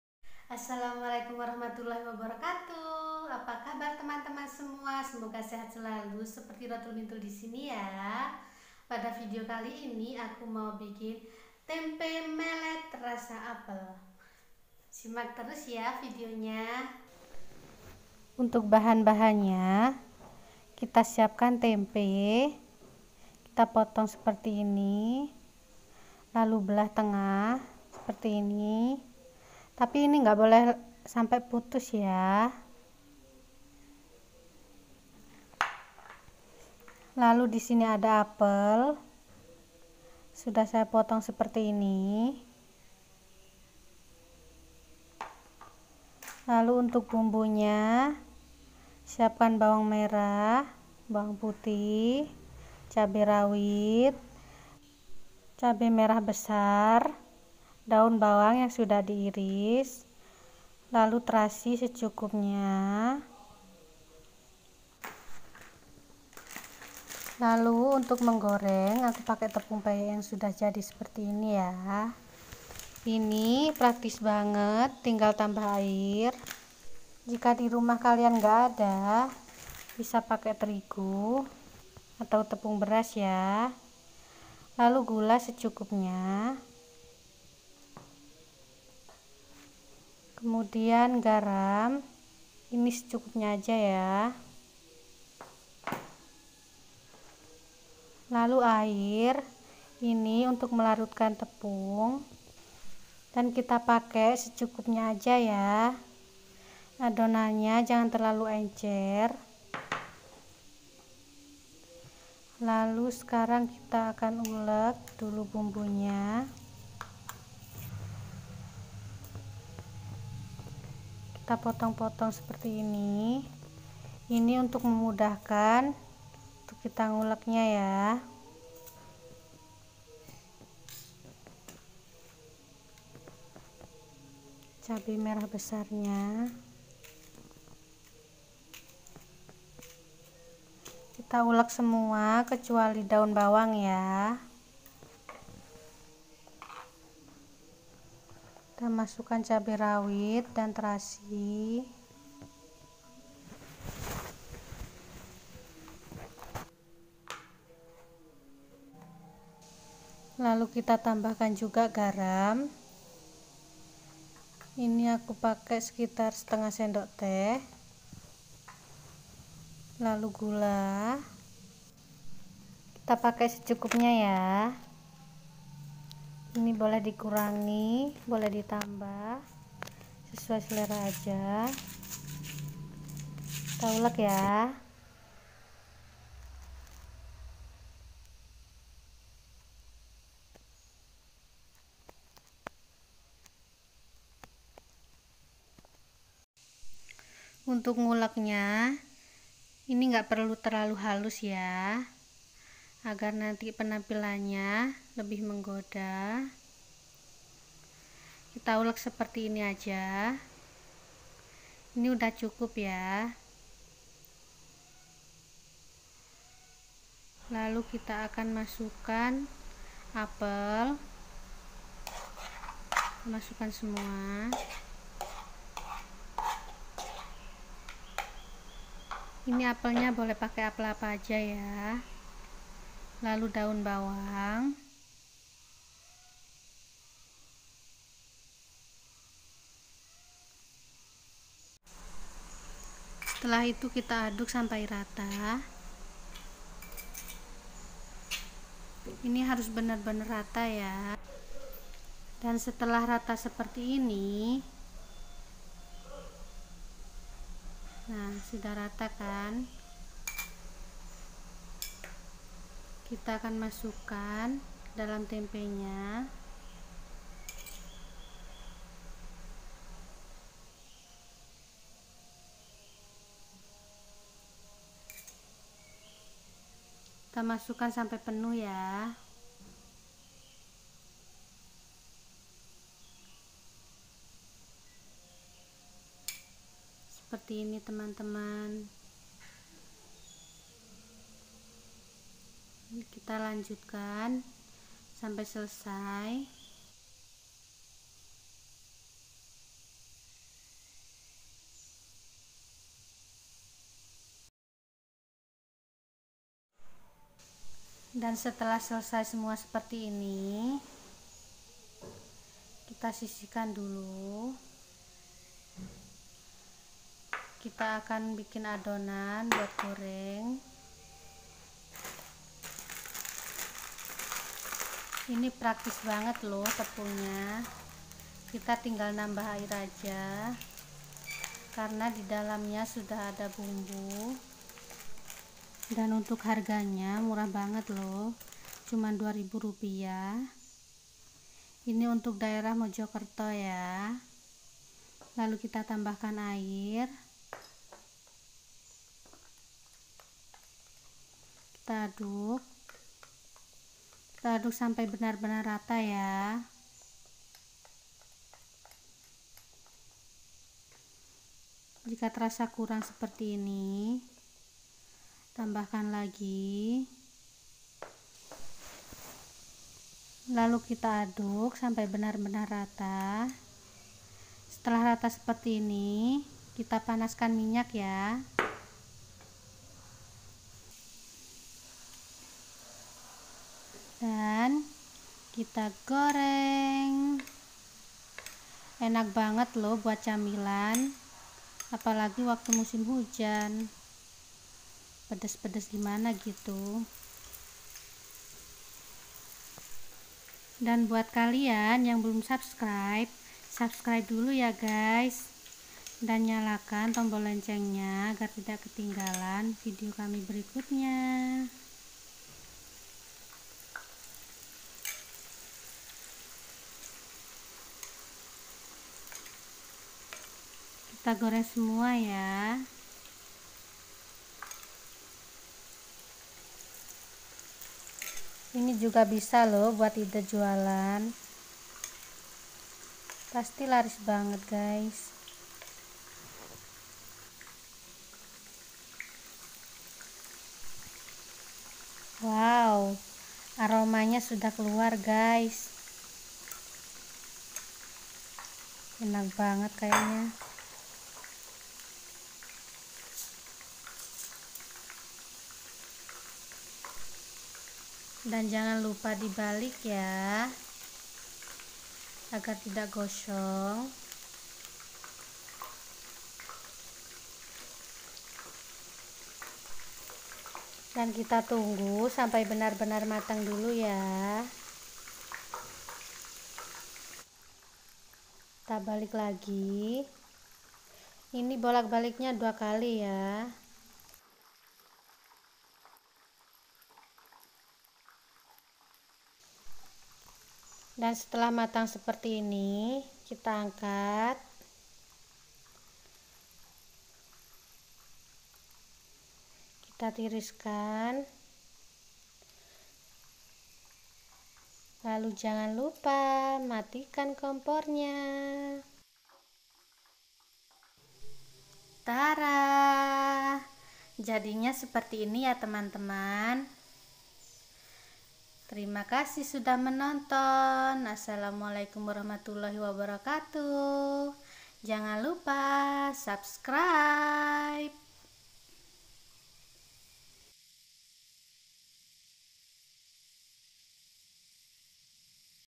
ide jualan || TEMPE MELET ALA ROTUL MINTUL
RESEP DAN CARA MEMBUAT TEMPE MELET RASA APEL || IDE JUALAN ALA ROTUL MINTUL kereasi olahan tempe yang bisa untuk ide jualan sampai saat ini di luaran belum ada yang jualan seperti ini biasanya tempe melet atau tempe jaletot hanya isi sambel aja gaess agar lebih kekinian kita tambahkan buah apel  yang membuat rasanya lebih nikmat dan unik